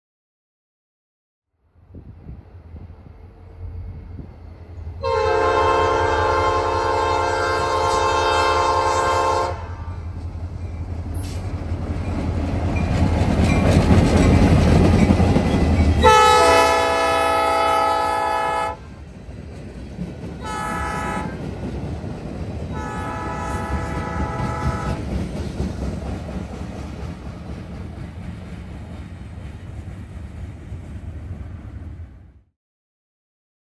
NS Dayton District-NS L04 EB Local @ CJ 193 W. Enon Ohio 7-26-2024
L04 will advance to the Enon Siding at CJ 190 and allow NS 179 WB to pass on the main.